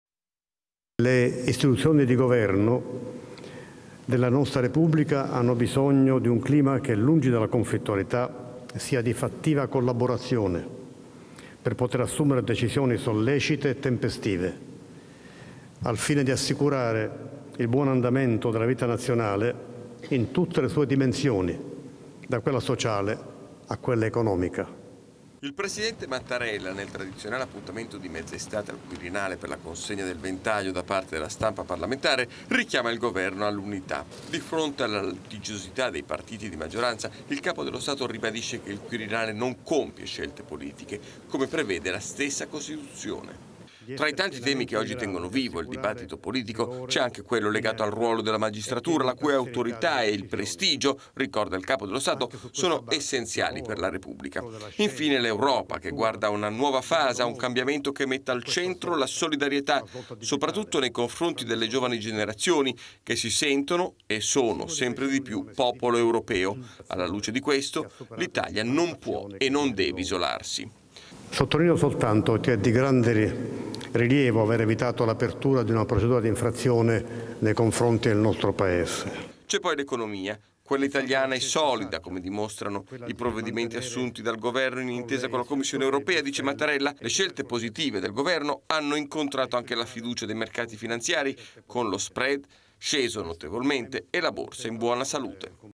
La cerimonia del Ventaglio, Mattarella: da istituzioni serve clima di collaborazione
la tradizionale cerimonia del Ventaglio, questa mattina al Quirinale. Le istituzioni hanno bisogno di un clima di fattiva collaborazione - l'appello del presidente Mattarella, dopo le liti e le divisioni che hanno agitato la politica nelle ultime settimane. Servizio di Leonardo Possati